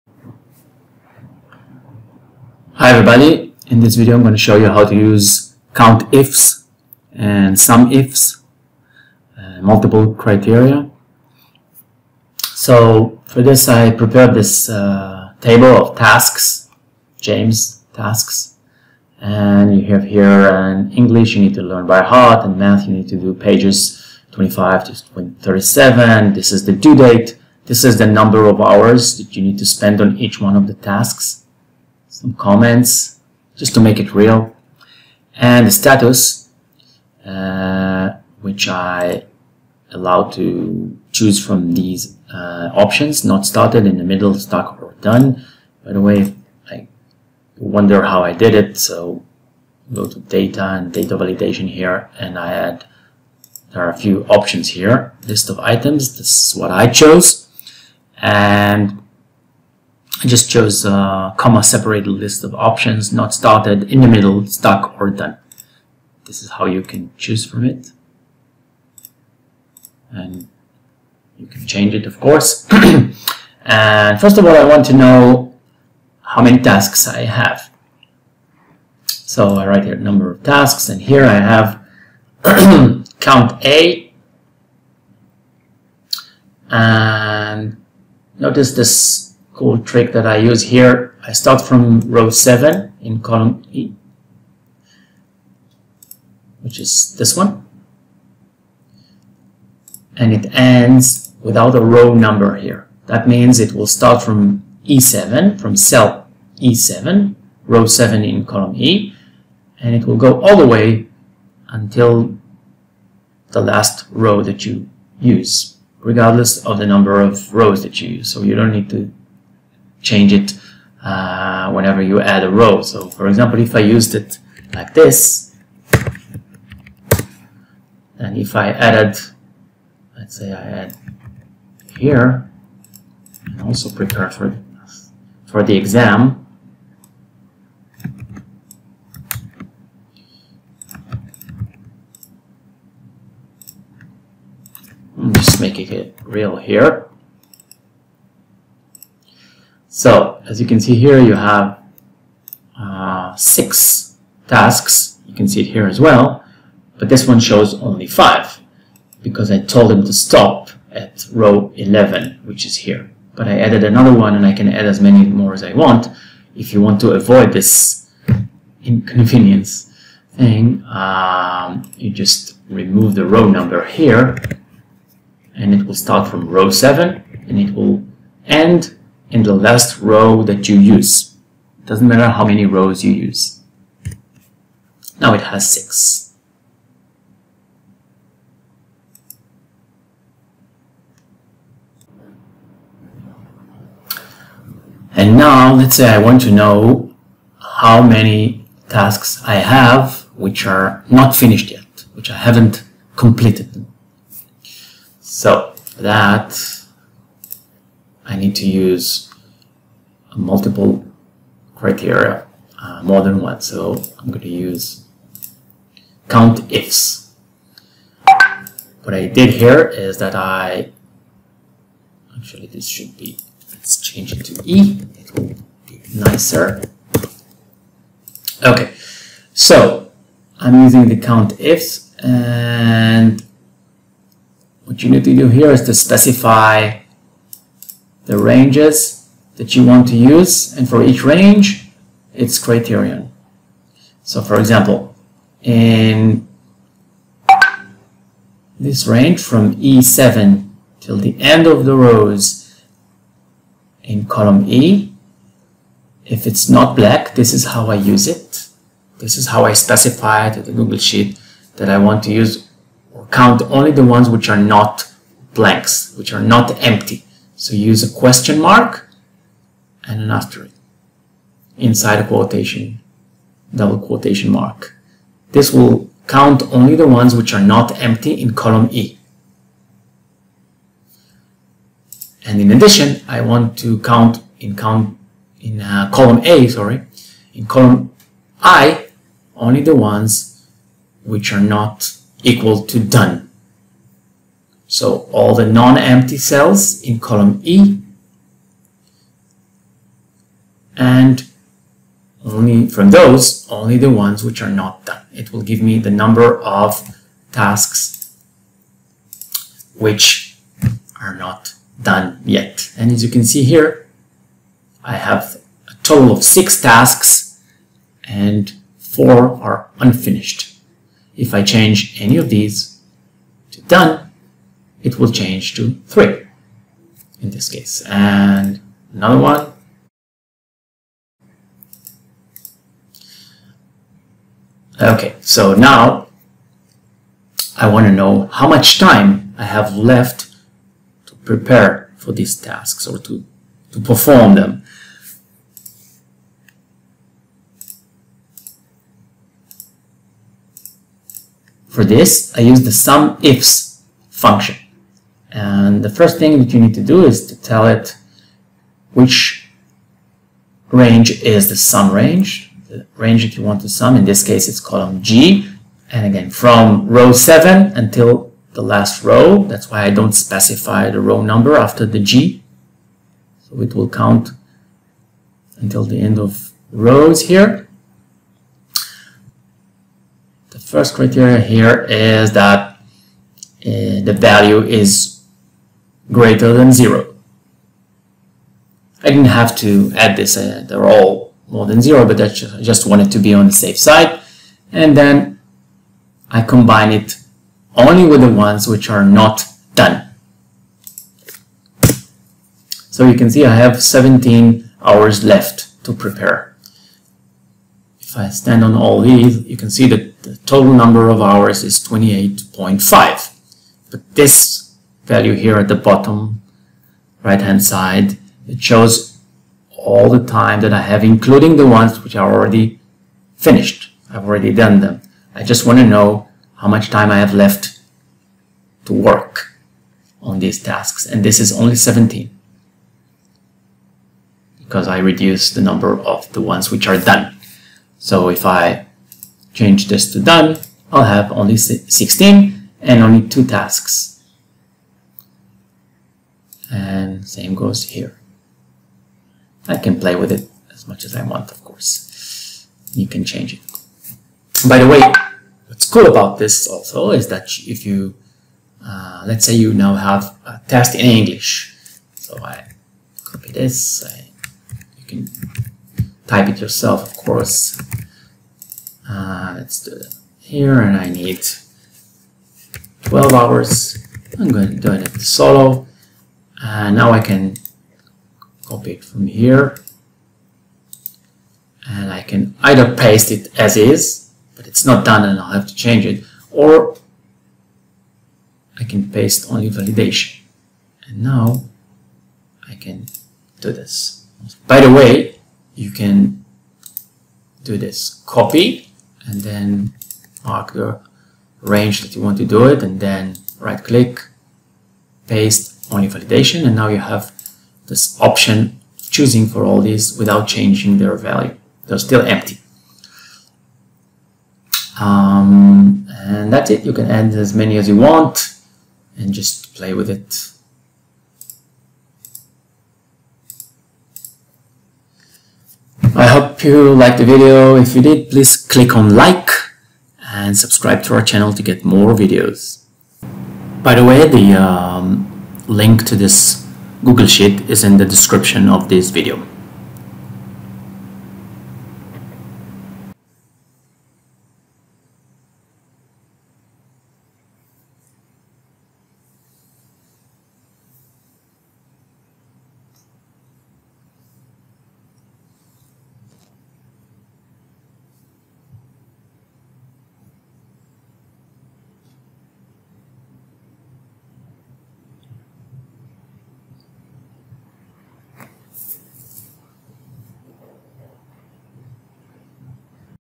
How to use COUNTIFs, SUMIFs and Data Validation in Google Sheets
In this video I teach how to use COUNTIFs, SUMIFs and Data Validation in Google Sheets
This video uses examples with multiple criteria.

Btw, do you want to learn how to solve a Rubik's cube 3 X 3 with new techniques and faster algorithms ?